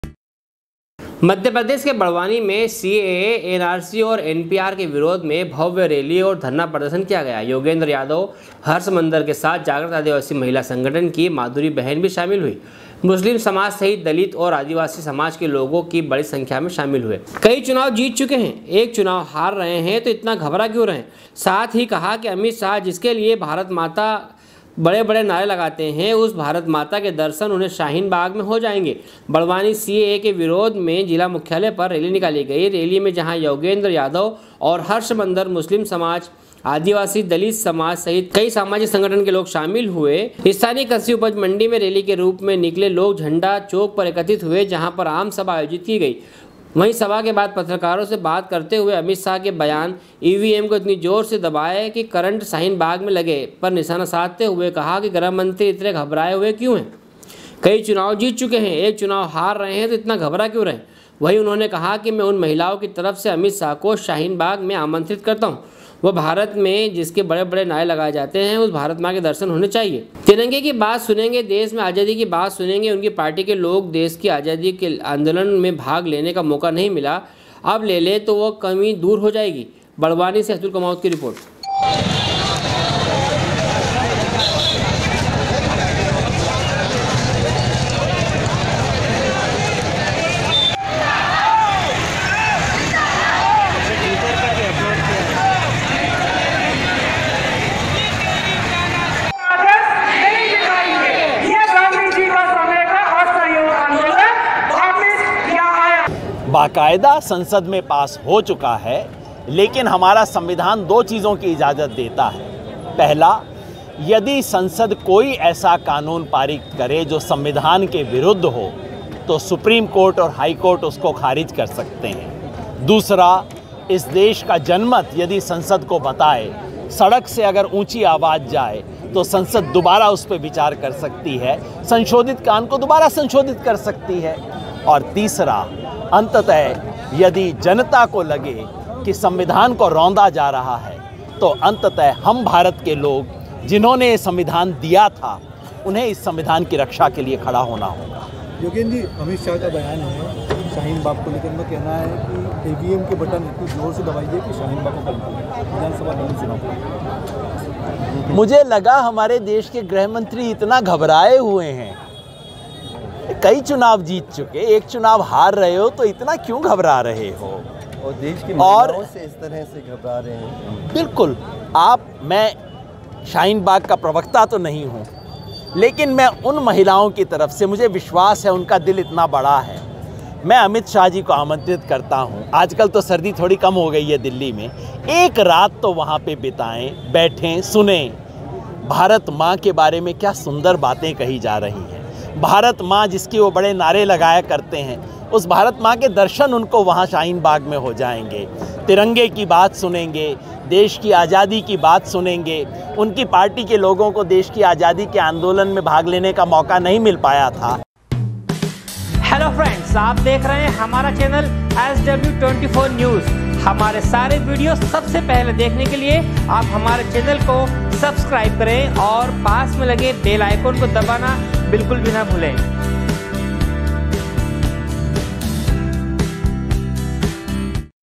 sw24news barwani, 27.01.2020
बड़वानी में सीएए के विरोध में भव्य रैली और धरना प्रर्दषन किया गया, योगेंद्र यादव हर्ष मंदर के साथ जाग्रत आदीवासी संगठन की माधुरी बहन भी शामिल हुई ,मुस्लिम समाज सहित दलित और आदिवासी समाज के लोग बड़ी संख्या में शामिल हुए थे बड़वानी से अतुल कुमावत की रिपोर्ट#sw24news